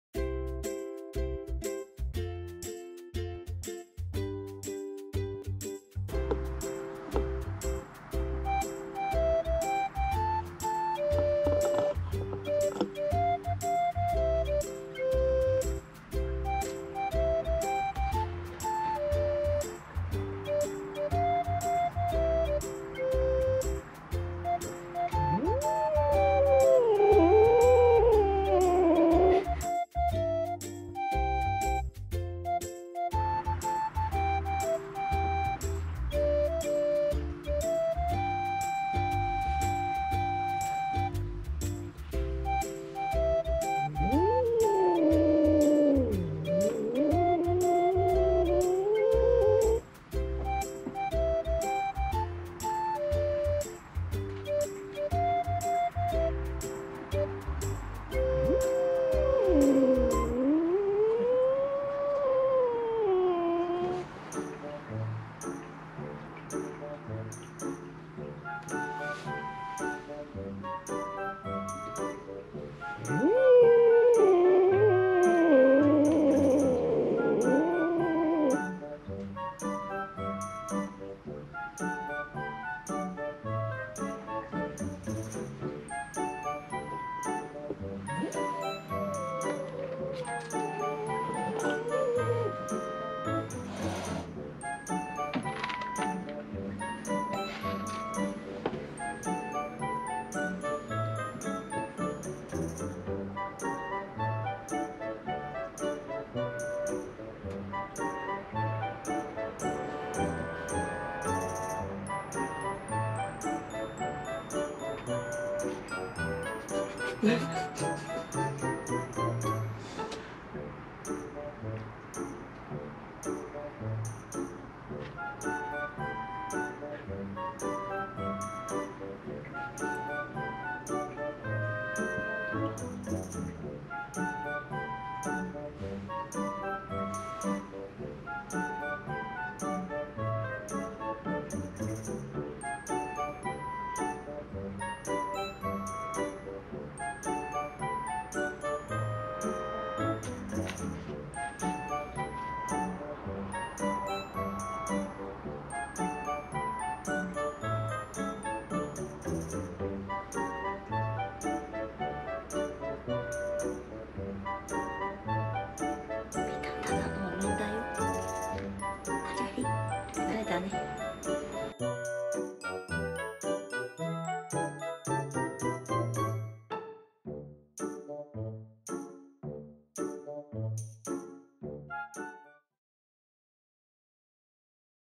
【シャー】恐る恐るお面に触ってみる猫!ピクシーこれは本当に怖かったよね。ごめん。
お面を触れられるようにしてみたけど
「シャー」が出てしまった．．．

ピクシーはこの前１歳になった男の子のネコ
ゆきは都内で看護師をしている女子

毎日日記として猫動画を上げていきますね！
皆さま応援よろしくおねがいします！